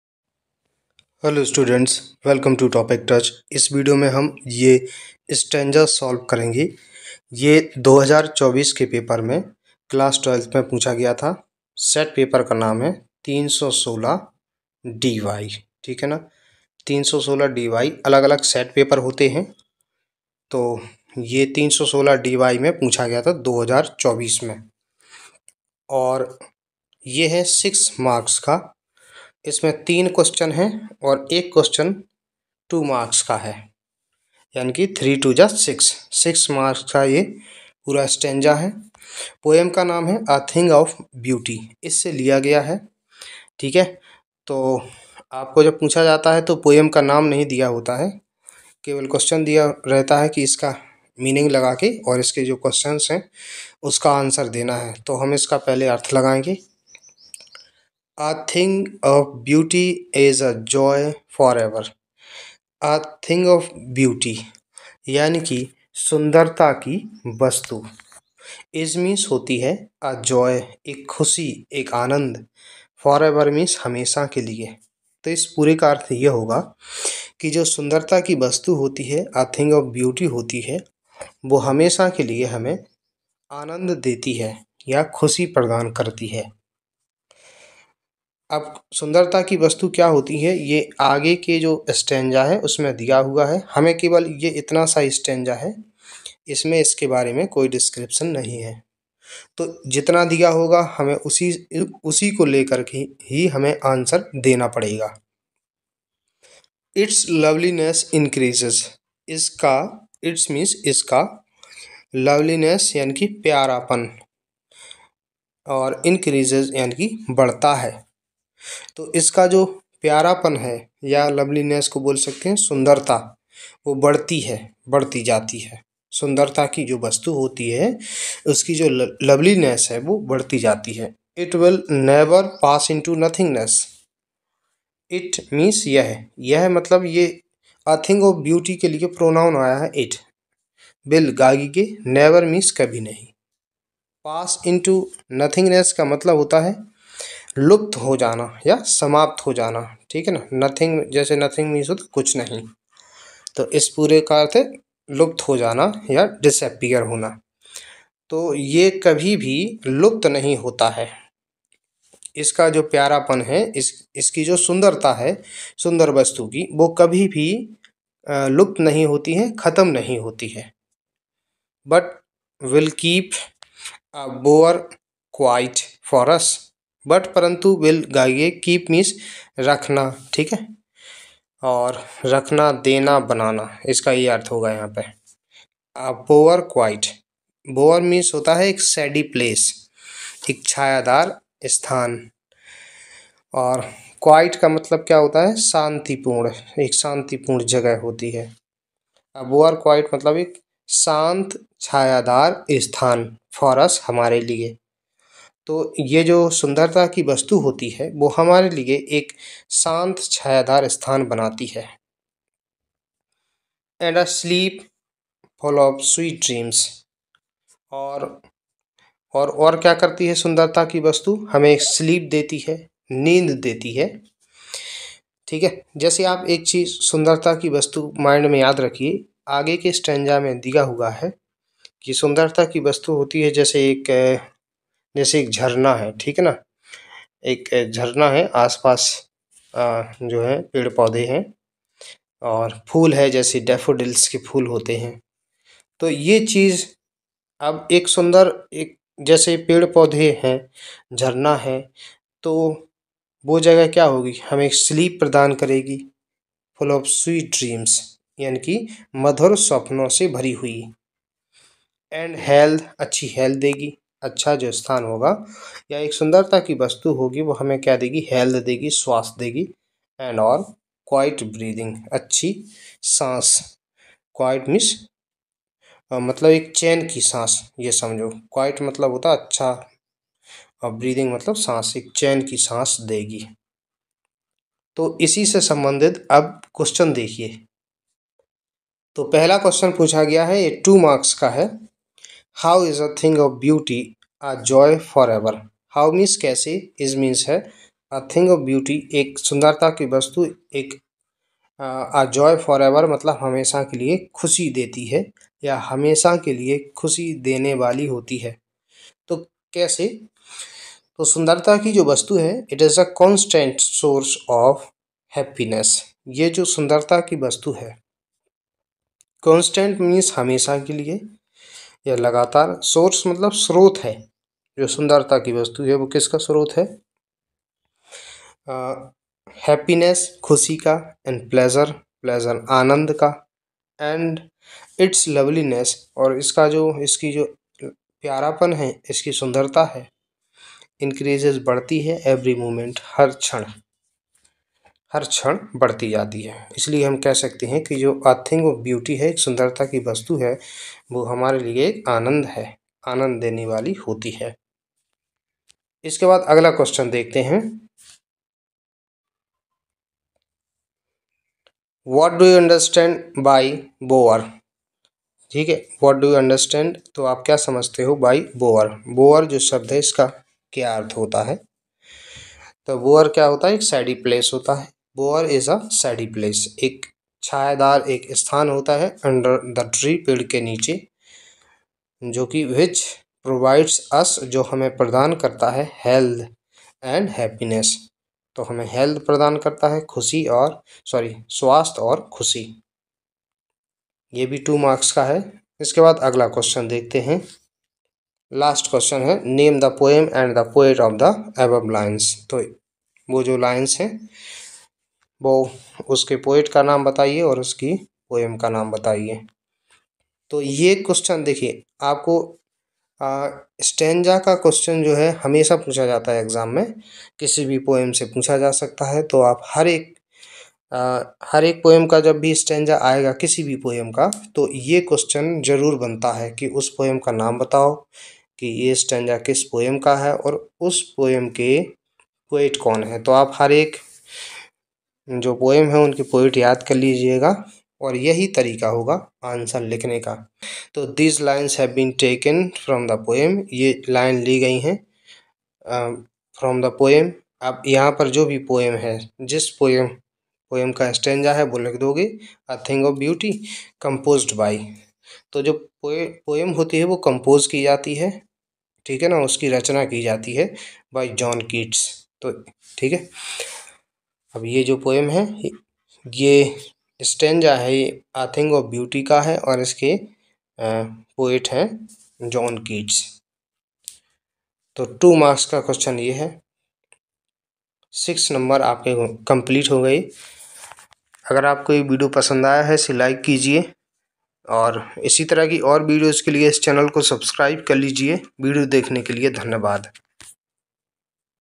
How is a thing of beauty a joy forever? answer the following questions || meaning of bower
class 12th 316 dy English paper 2024 solution
Read the following Extract carefully and answer the questions that follow:-
Stanza: A Thing of Beauty
A thing of beauty is a joy forever
Its loveliness increases, it will never
Pass into nothingness; but will keep
A bower quiet for us, and a sleep
Full of sweet dreams, and health, and quiet breathing.

a) How is a thing of beauty a joy forever?

b) What do you understand by bower?

c) Name the poem and the poet of the above lines.